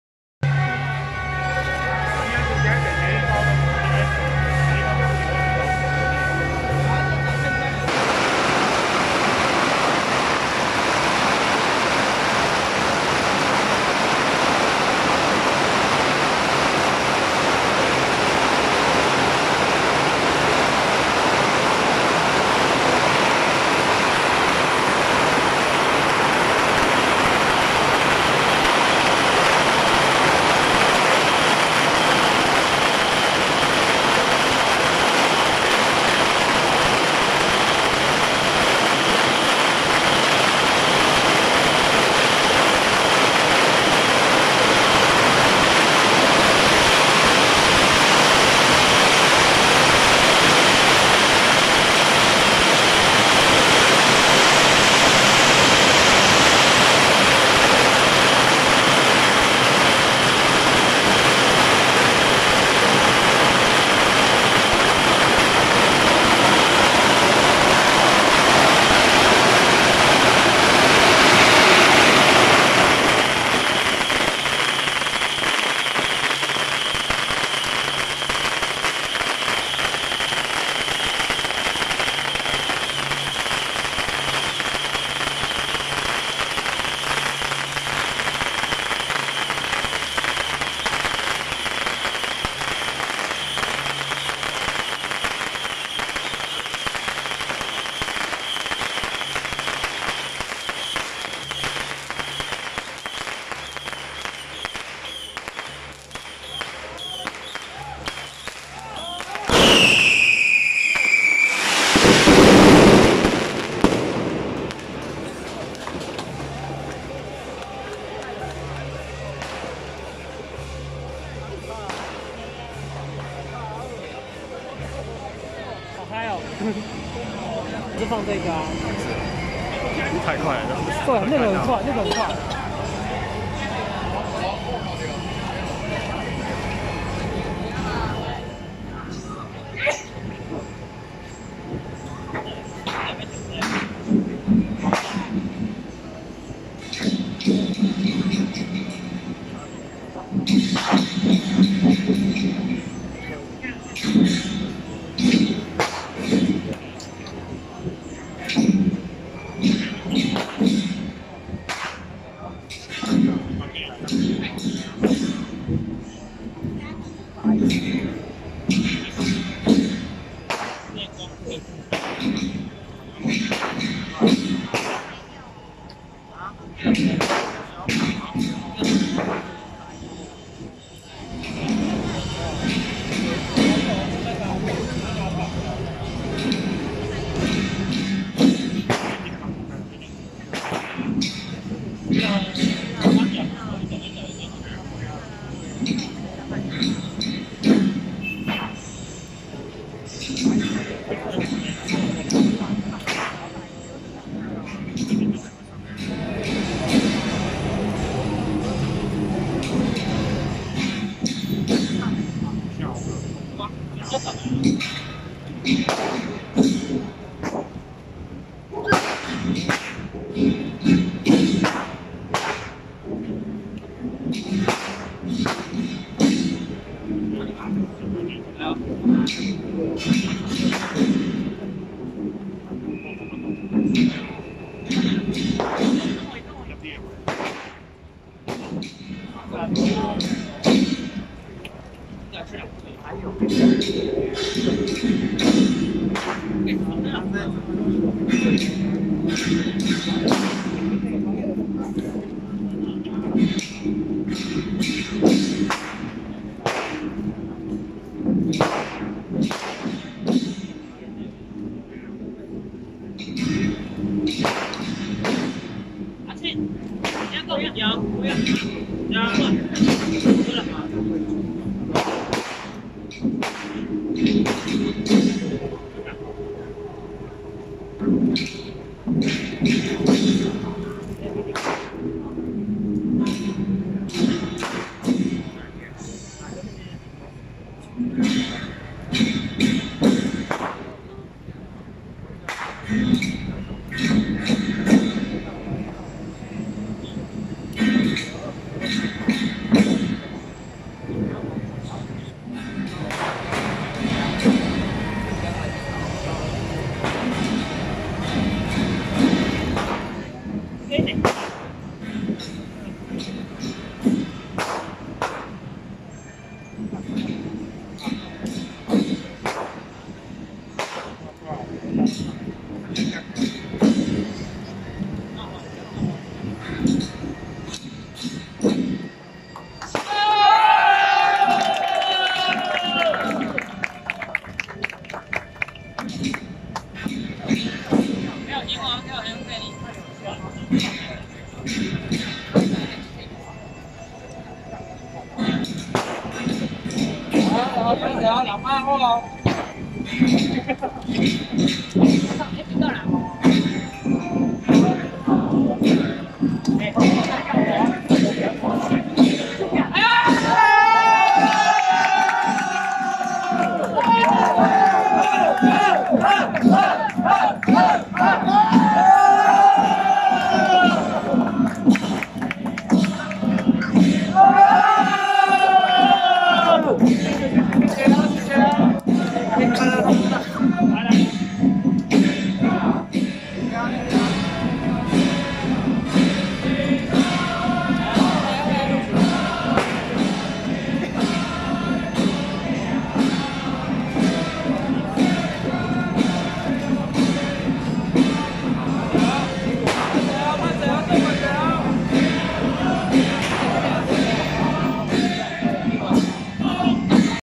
【高雄水門紫雲堂 炮贊 中和游公舘-關聖帝君 聖駕】~108中和游公舘關聖帝君開光回駕遶境
高雄水門紫雲堂 炮贊 中和游公舘-關聖帝君 聖駕